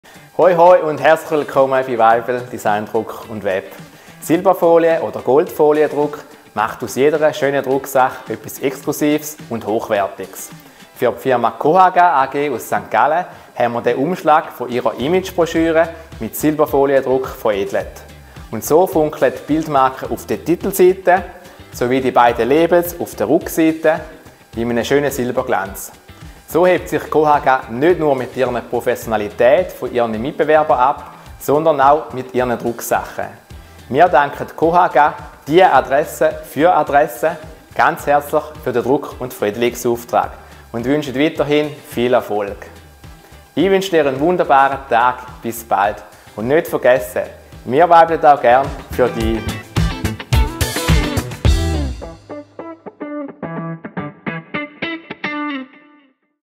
Broschüren mit Silberfoliendruck
Silberfoliendruck oder Goldfoliendruck macht aus jeder schönen Drucksache etwas Exklusives und Hochwertiges.

Für die Firma cohaga AG aus St.Gallen haben wir den Umschlag ihrer Imagebroschüre mit Silberfoliendruck veredelt. Und so funkelt die Bildmarke auf der Titelseite sowie die beiden Labels auf der Rückseite im schönen Silberglanz.

So hebt sich cohaga nicht nur mit ihrer Professionalität von ihren Mitwerbern ab, sondern auch mit ihren Drucksachen.

Wir danken cohaga – die Adresse für Adressen – ganz herzlich für den Druck- und Veredelungsauftrag und wünschen weiterhin viel Erfolg!

Wir weibeln für deinen Erfolg!

Weibel Druck & Design AG
Grafik | Druck | Web | Film

«Wir wollen die beste Druckerei der Schweiz werden!»
Dani Weibel, Inhaber